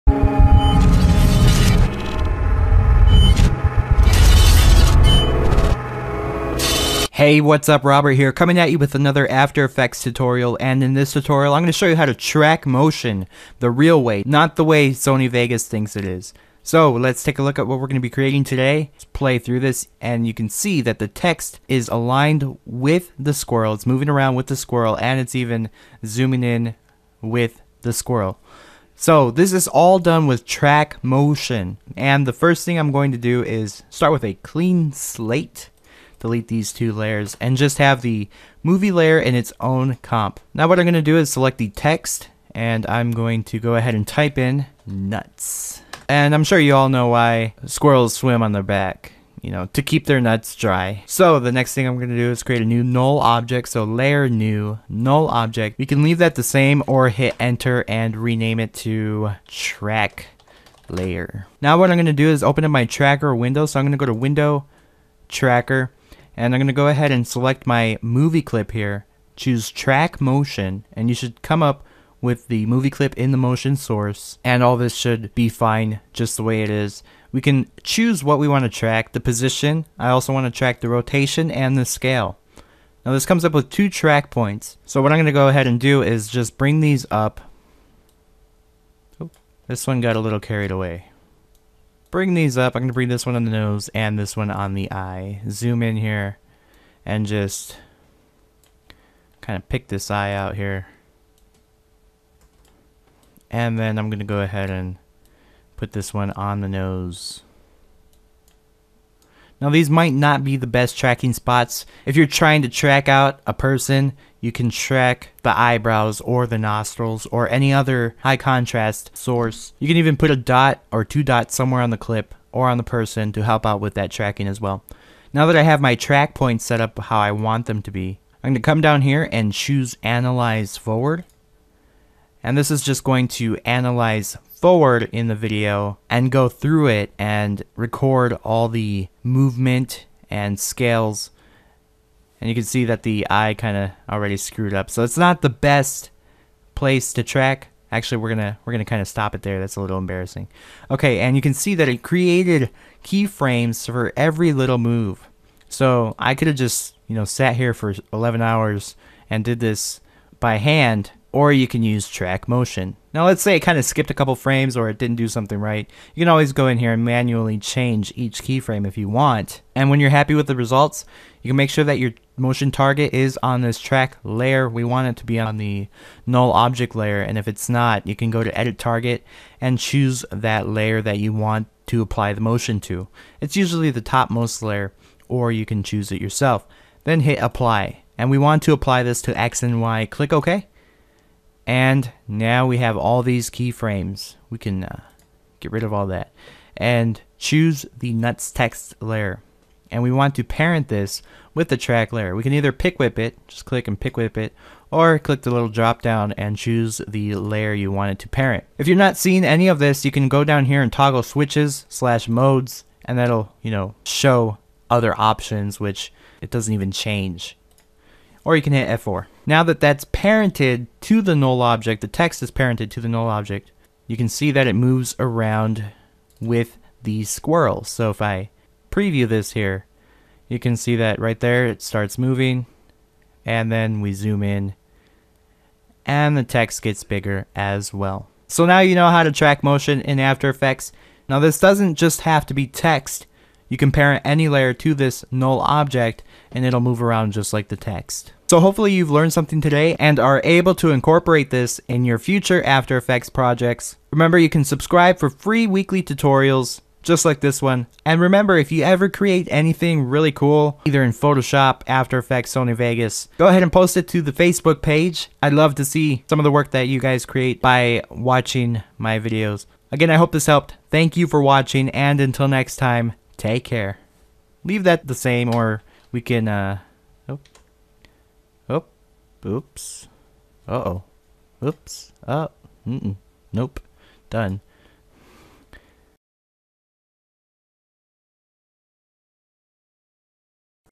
After Effects - Track Motion
In this basic tutorial, I show you how to track motion in After Effects CS5.